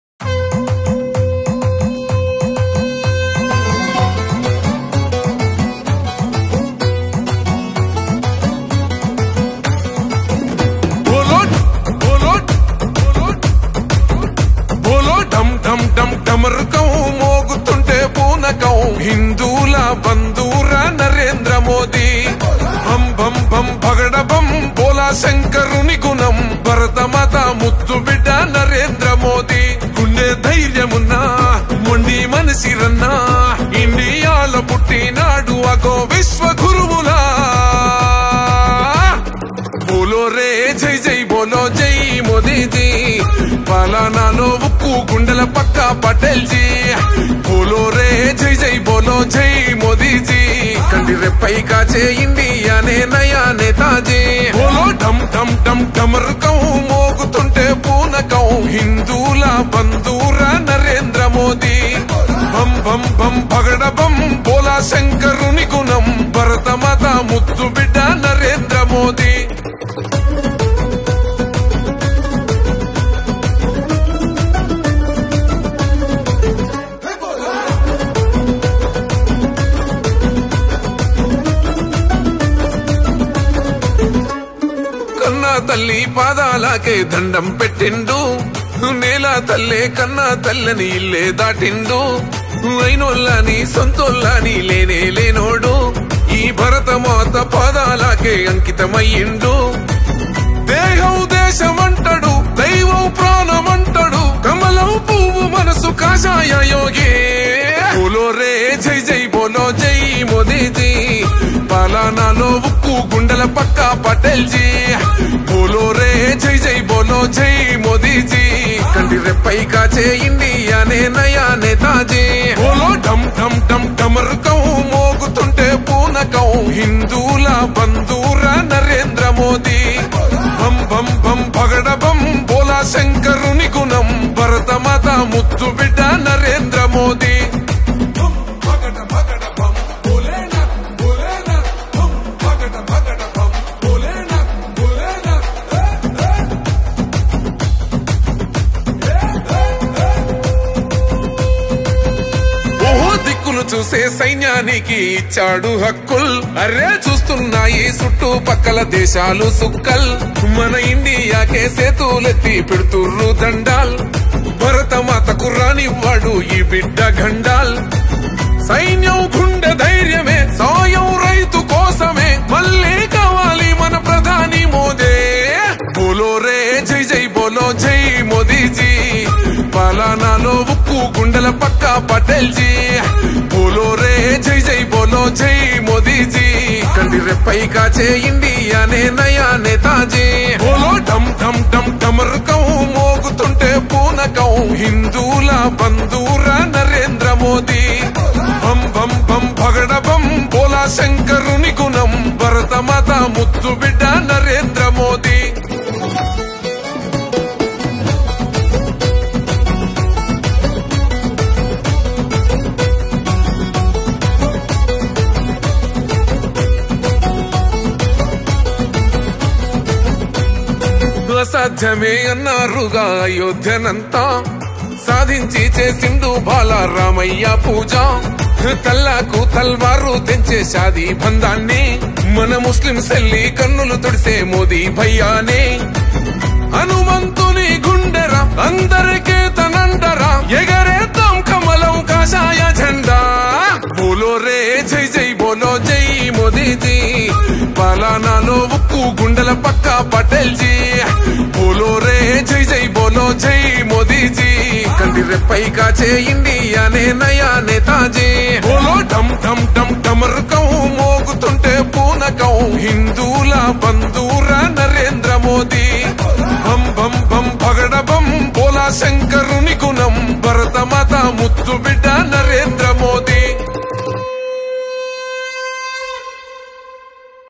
Nallagonda gaddhar narsanna song.నరేంద్ర మోడీ పై నల్లగొండ గద్దర్ అద్భుతమైన పాట.@LocalTv587
నల్లగొండ గద్దర్ నరసన్న నరేంద్ర మోడీపై పాట.@LocalTv587